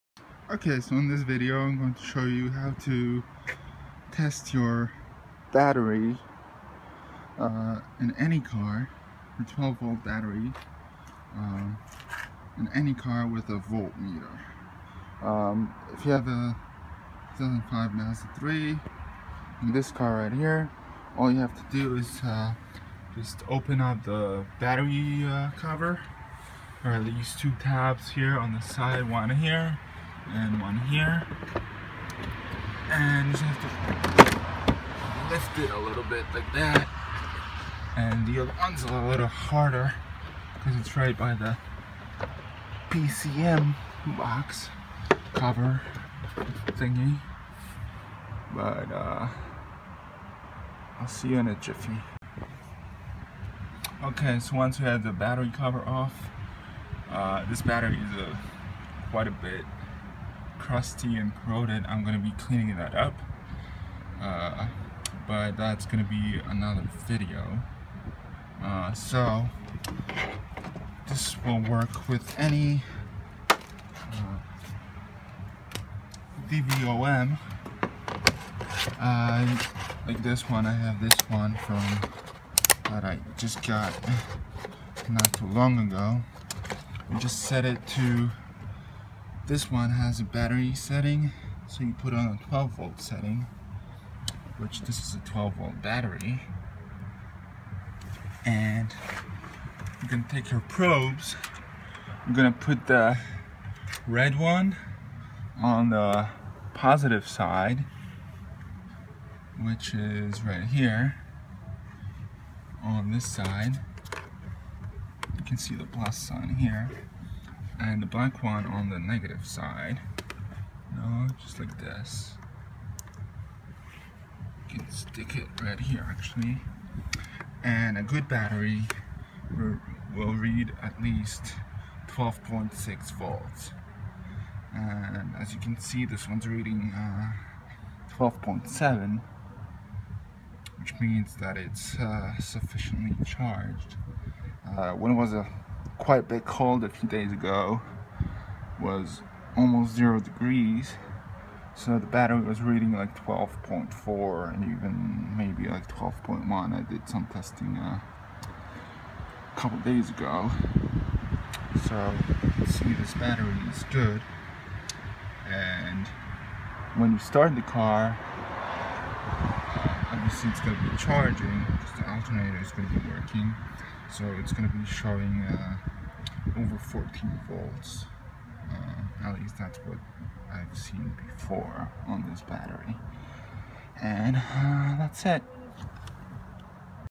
How to test a car battery
This video shows how to test a 12V car battery using a digital multimeter. I for got to mention in the video that before testing the battery you should turn on your high beams for about 15 seconds then turn them off and wait about 2 minutes.  This will remove the battery's surface charge. Make sure that the engine and all accessories are turned off and the key is out of the ignition before testing the battery. This will ensure that your battery voltage reading is accurate. The battery voltage of a fully charge battery should be 12.6 or slightly higher.